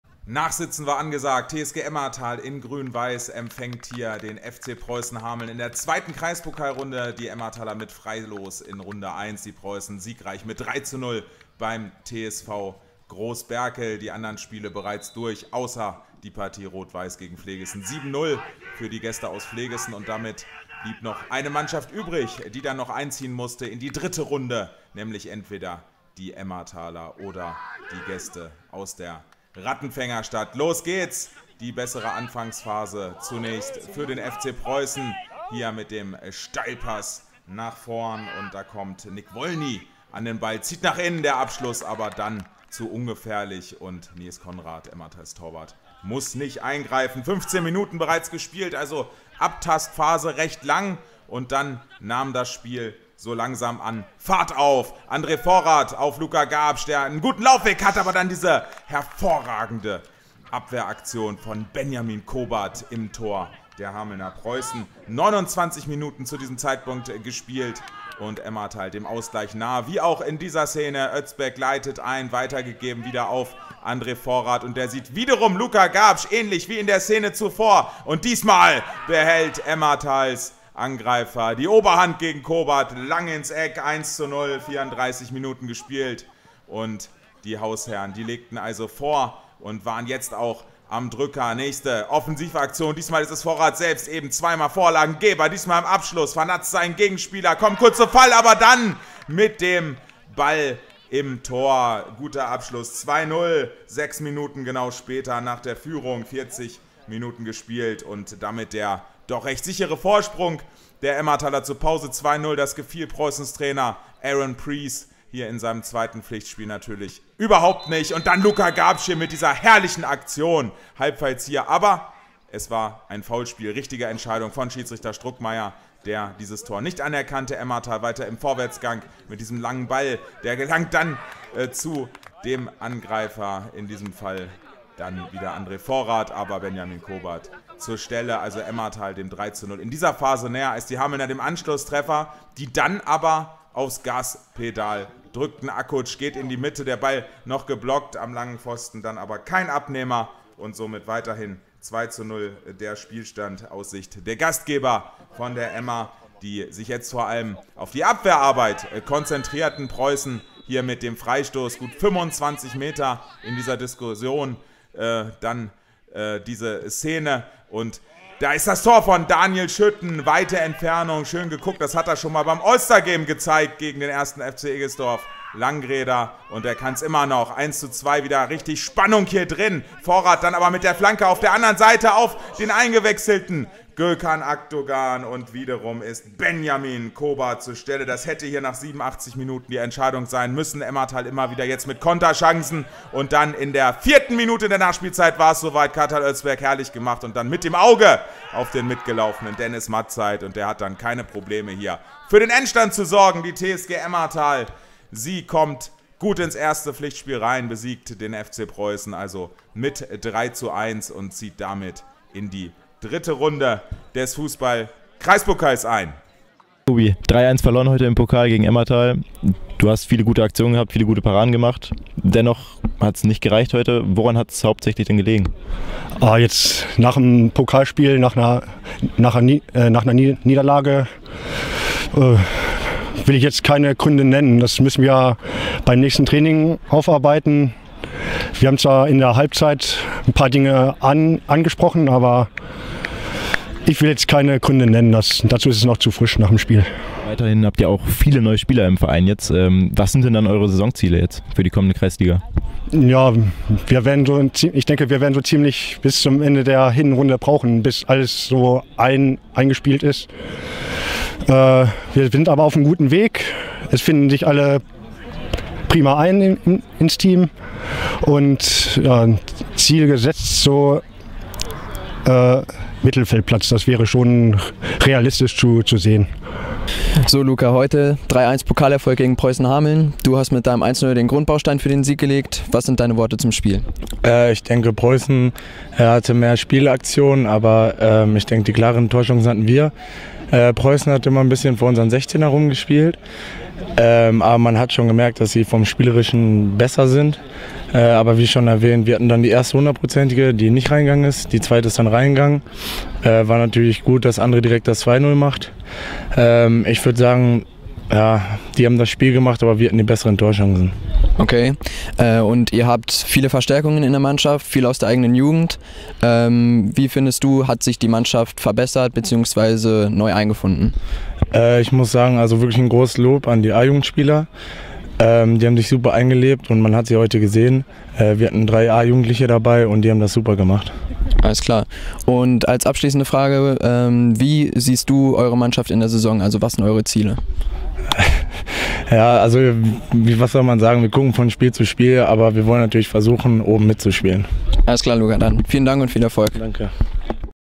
Freistoß aus 25m: Emmerthal - Preussen Hameln
Im Kreispokal kam es gestern zum Duell zwischen der TSG Emmerthal und dem FC Preussen Hameln. Neben einem Freistoß aus 25 Metern Entfernung gab es drei weitere Tore sowie einige sehenswerte Torchancen zu bestaunen.